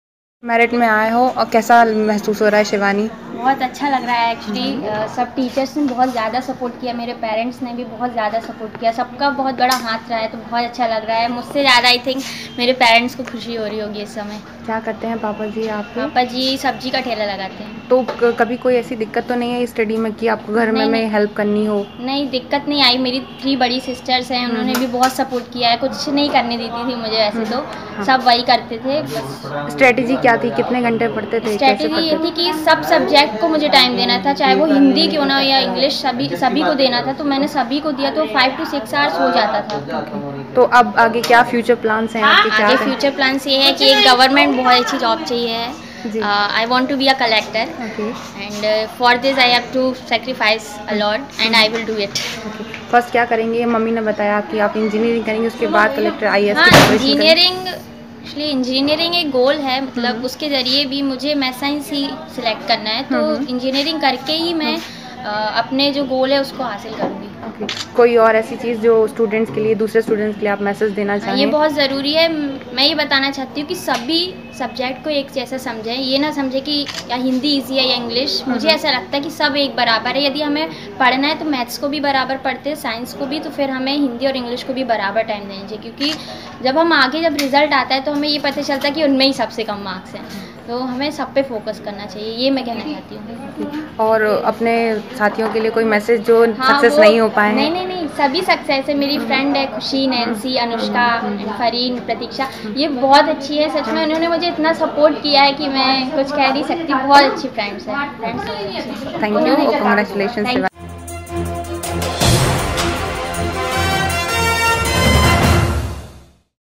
MP Board 10th result 2017 | 10th Topper Shivani sahu exclusive Interview
MP Board 10th result 2017 | 10th Topper Shivani sahu exclusive Interview at Jabalpur Madhya Pradesh